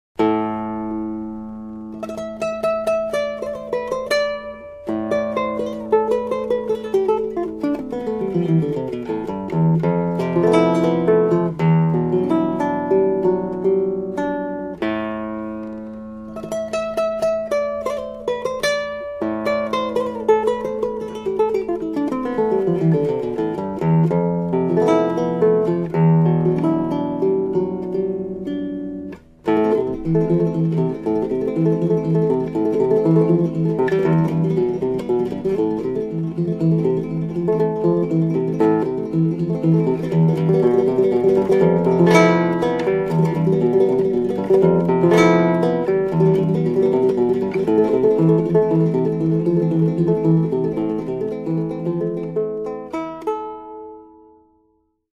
Boaz Nylon String Travel Guitar Indian/Spruce at Dream Guitars
This is a great guitar for practicing when you can't carry a full-sized instrument. The neck feels just like it's big brother. I wish I had one of these for my many hotel stays. I always bring a guitar and this would be so easy to put in an overhead and it sounds great acoustically. No need for headphones.